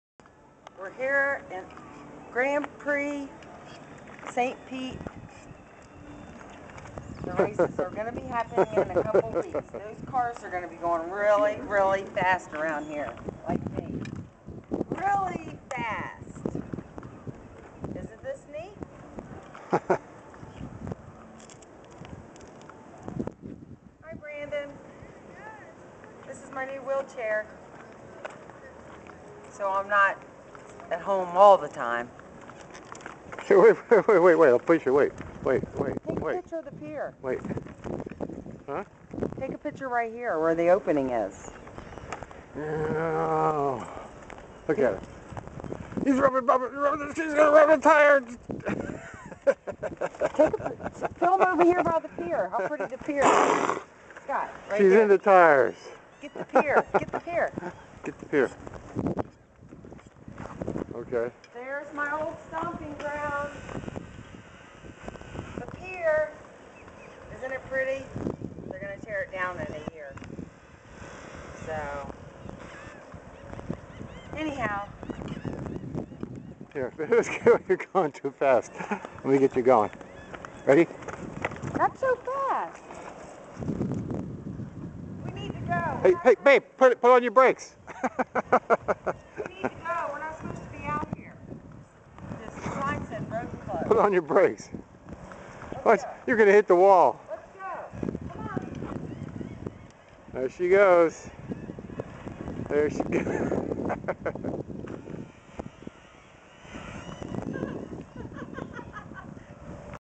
christinas gran prix
on the track in front of the Dali museum  setting up for the race in 2 weeks